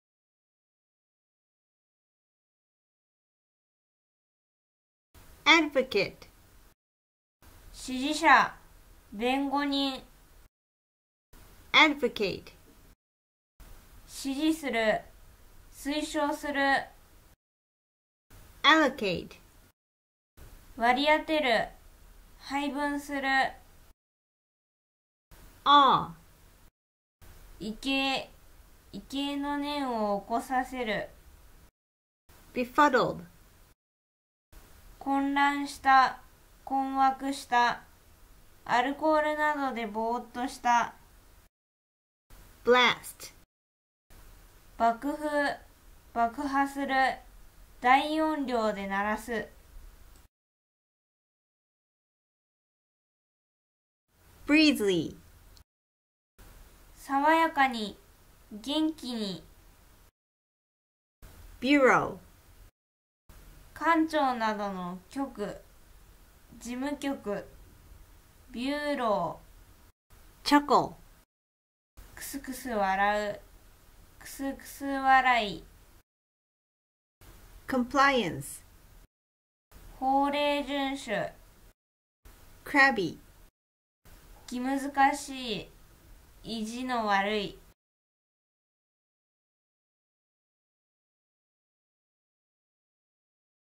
英単１５３ 英検準１級 【advocate allocate awe befuddled blast breezily bureau chuckle compliance crabby 】英単語
【advocate allocate awe befuddled blast breezily bureau chuckle compliance crabby 】を読み上げます☺